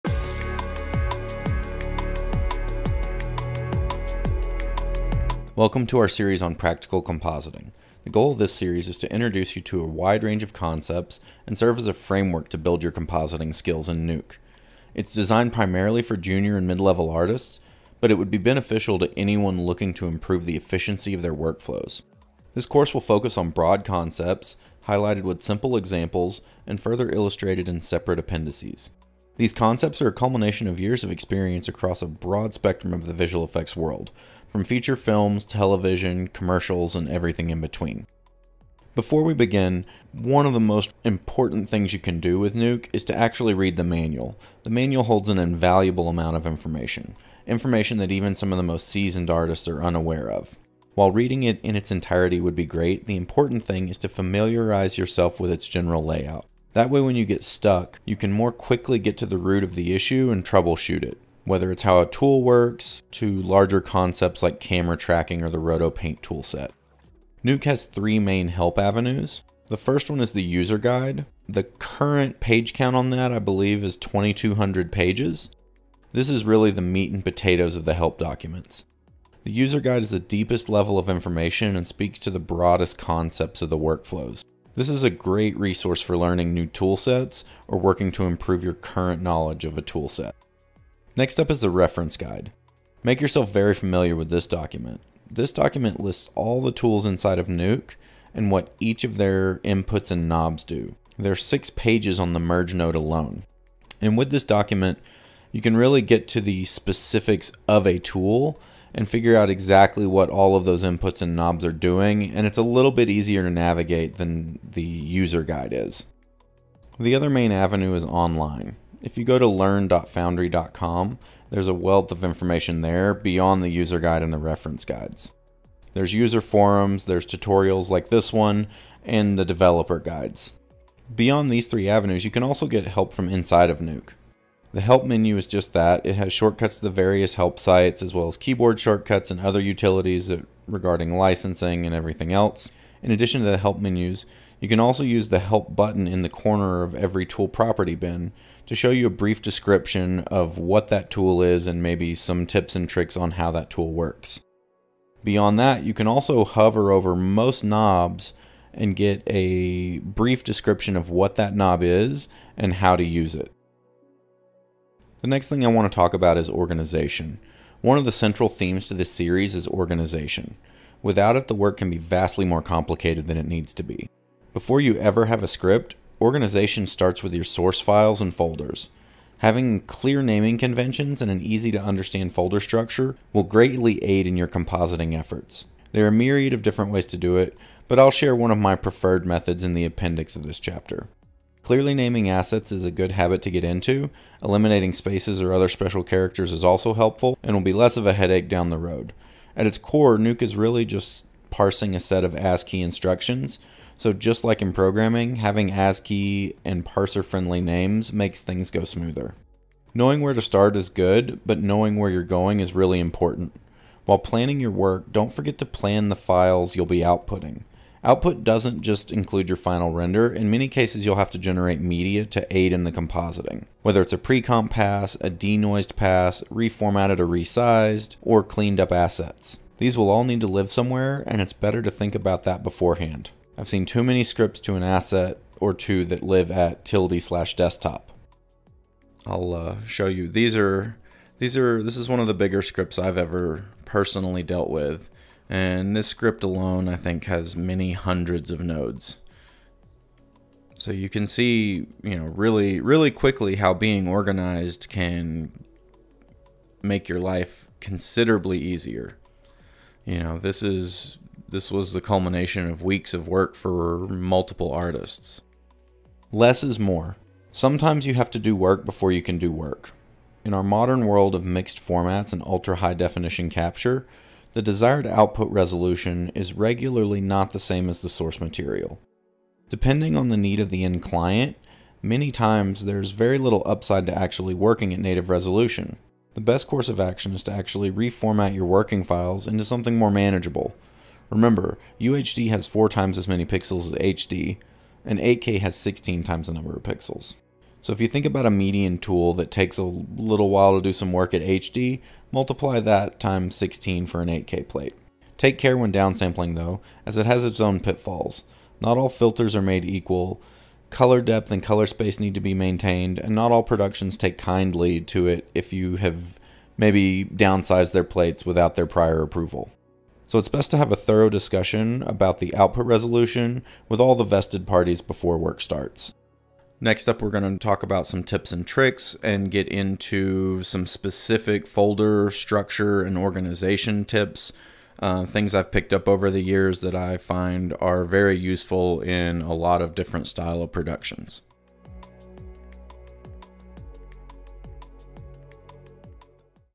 Before You Begin: 1. Read the Manual
Welcome to Austin Meyers' Nuke course. In this episode, Austin introduces the course and one of the most important tools you can use for Nuke, no matter your skill level.